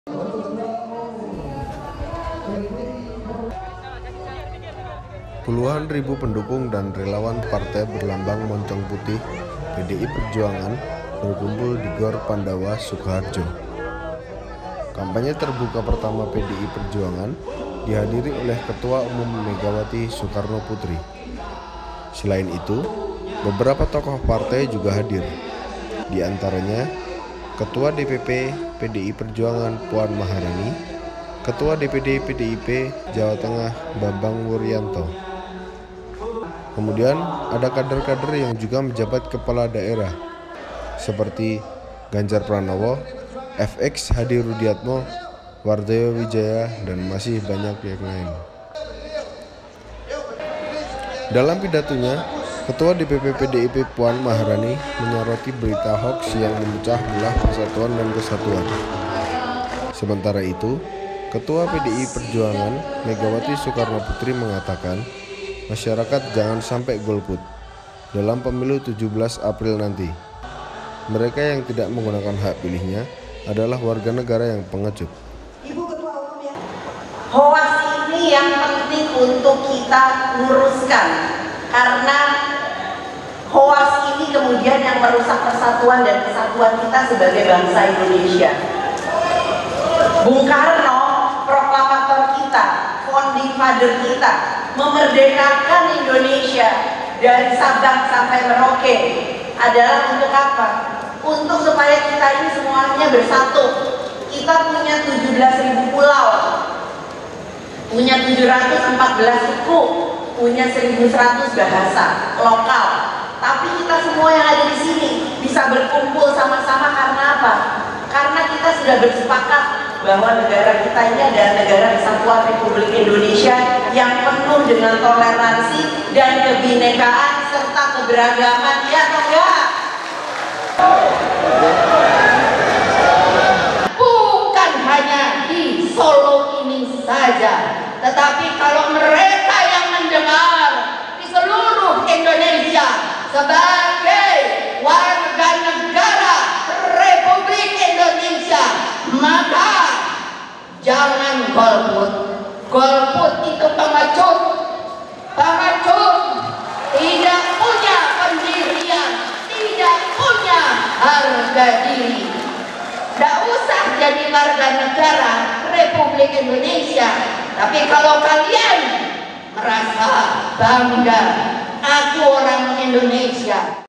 Megawati Soekarnoputi : Jangan Golput, Golput Itu Pengecut
SUKOHARJO – Rapat Umum Partai Demokrasi Indonesia Perjuangan (PDIP) Sukoharjo di GOR Pandawa Solo Baru, Kecamatan Grogol Minggu (31/3) dihadiri sang Ketua Umum Megawati Soekarnoputri.